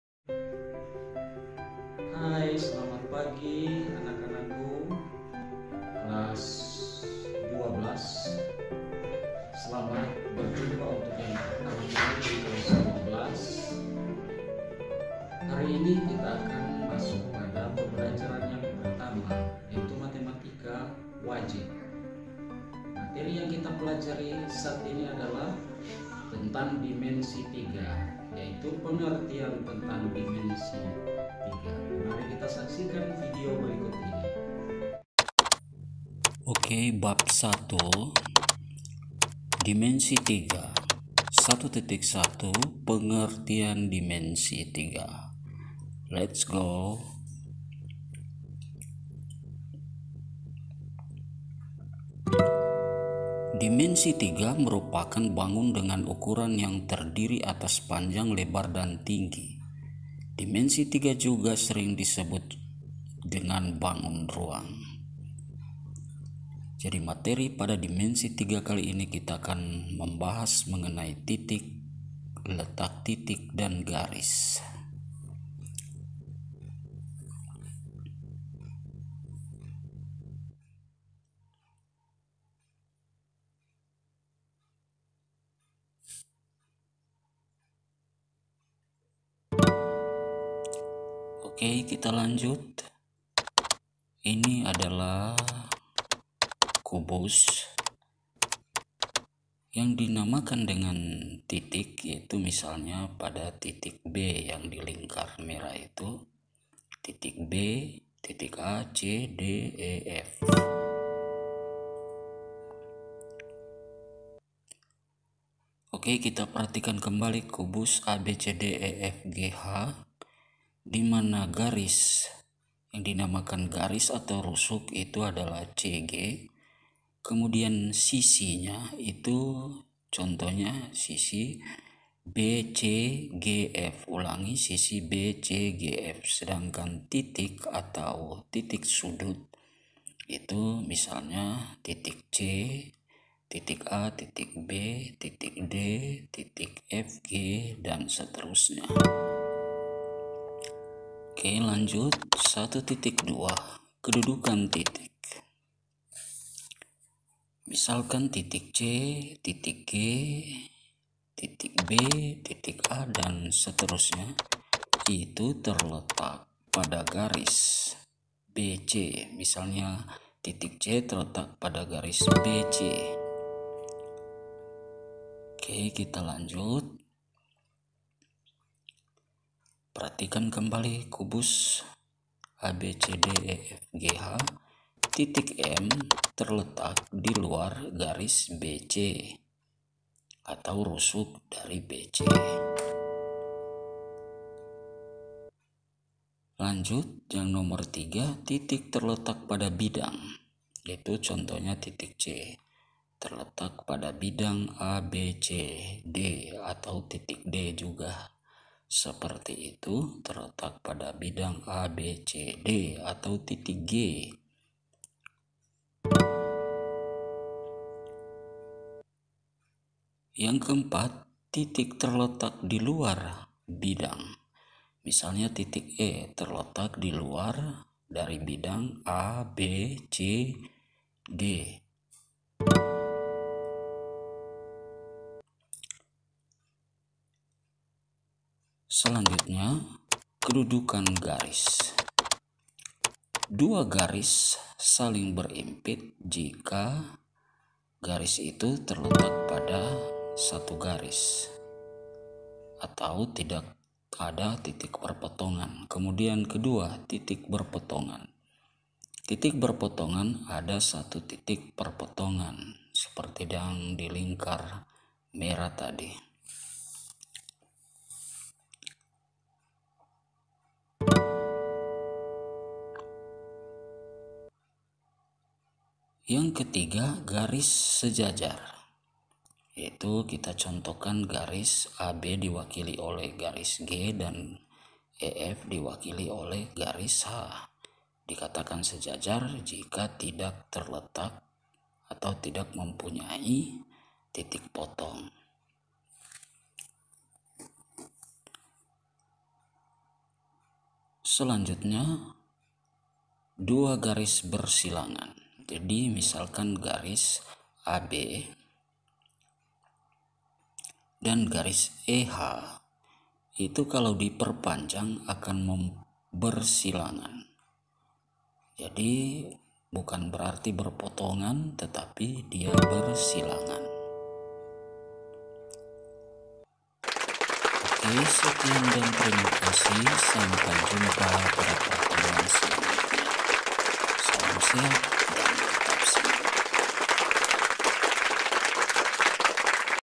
DIMENSI 3 || PENGERTIAN JARAK.PART-1
@mathth adalah wadah untuk mempelajari tentang matematika dasar. saat ini materi yang akan dipelajari adalah Dimensi tiga matematika wajib kelas XII SMA semester ganjil, tentang pengertian dimensi 3 dan unsur-unsut lainnya seperti kedudukan titik pada garis, bidang dan ruang. serta kedudukan garis yang saling berimpit, sejajar, berpotongan .

mathth matematikadasar dimensitiga semesterganjil.